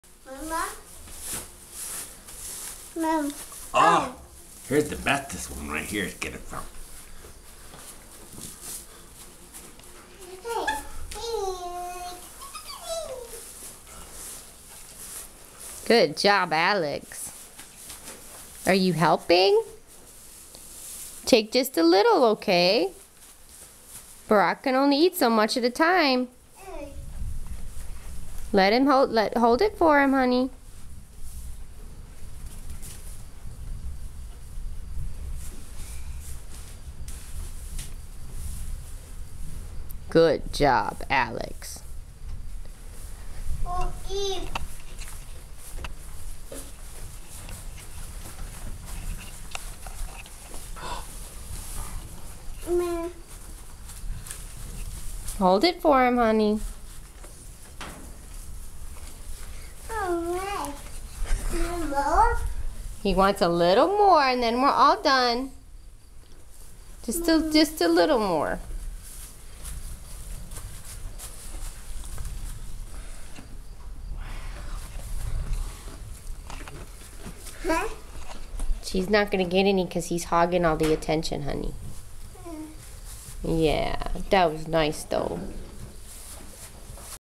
Alex and Barack, the goat
Alex feeds a goat on a small, Kentucky farm